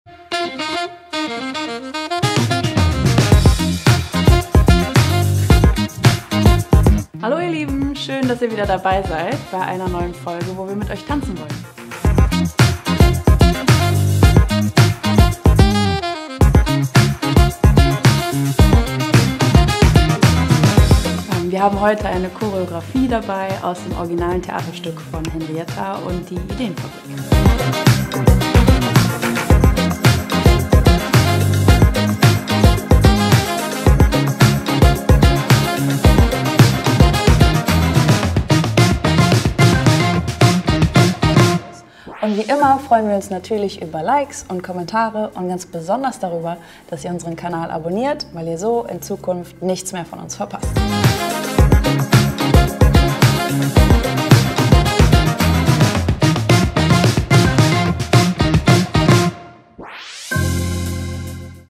Ideenfabrik - Choreo (Trailer)
Habt ihr Lust, die originalen Choreografien aus dem Theaterstück „Henrietta und die Ideenfabrik“ mit uns zu tanzen? Dann klickt euch durch unsere YouTube-Reihe und lernt Schritt für Schritt, wie die Profis zu performen. Wir freuen uns auf euch!